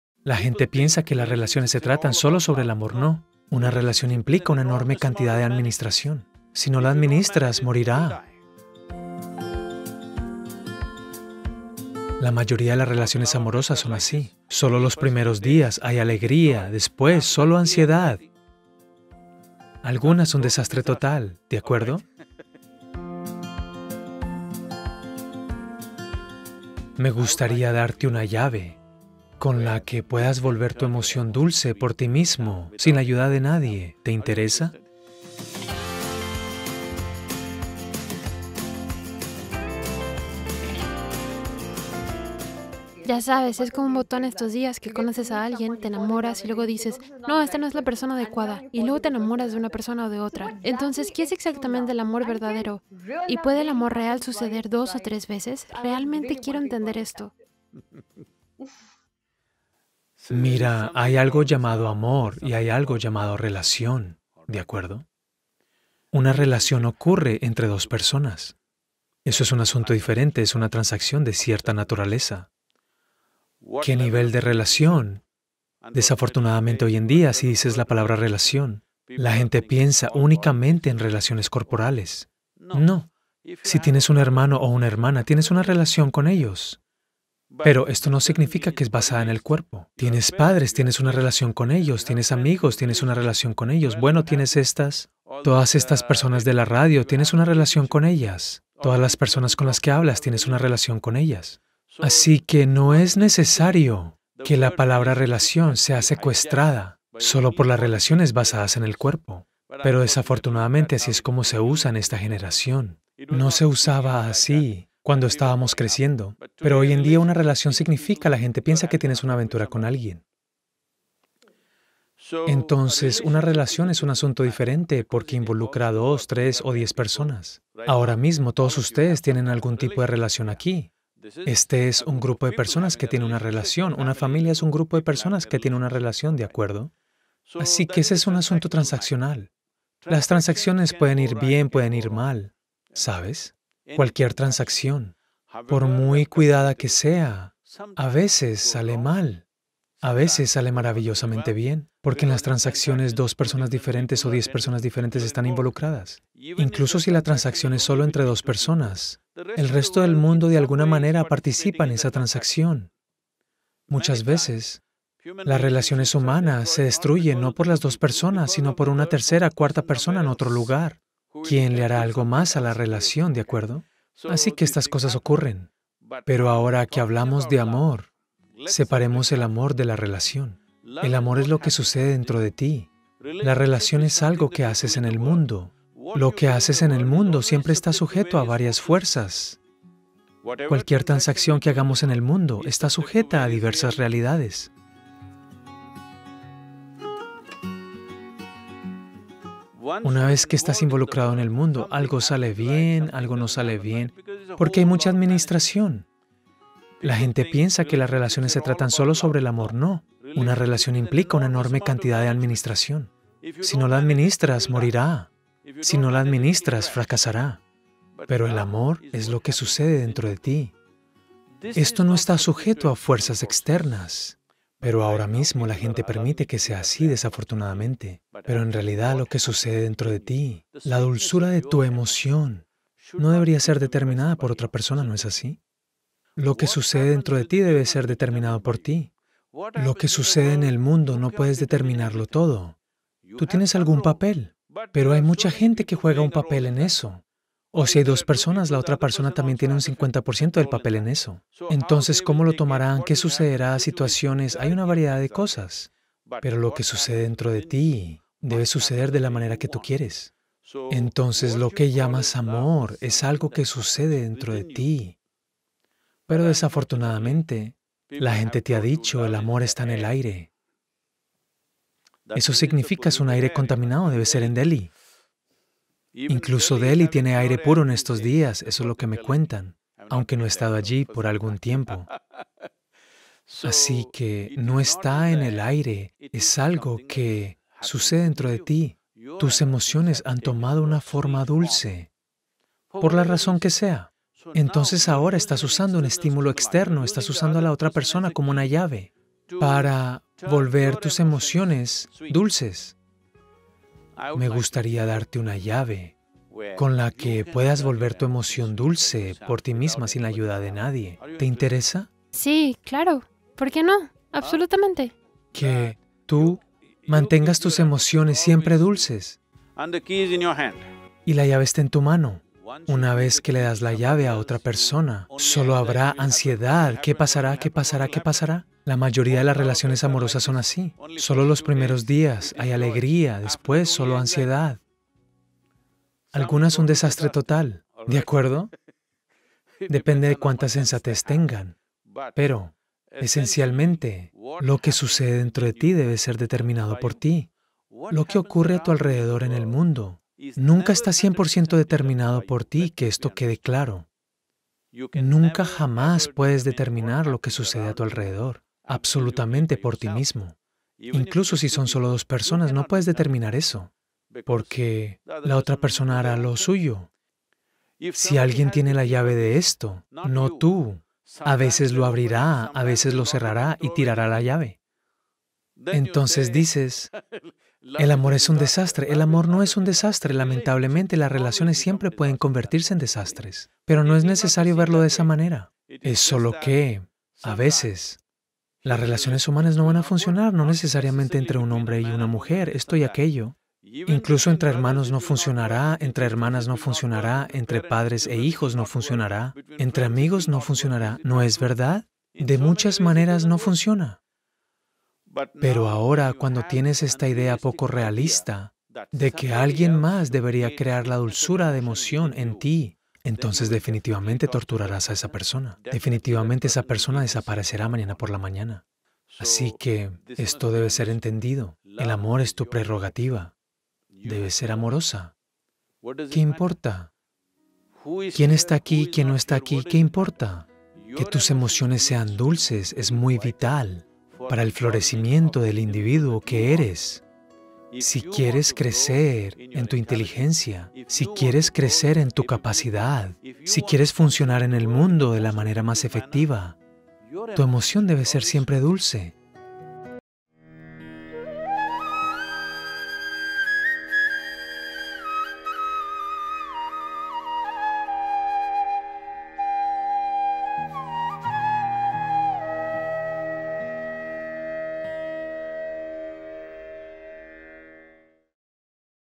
Sadhguru revela el secreto para el amor verdadero
Sadhguru responde a una pregunta sobre cómo encontrar el verdadero amor, y describe la diferencia entre el amor y las relaciones, mientras explica cómo tener emociones dulces es necesario para que un individuo florezca.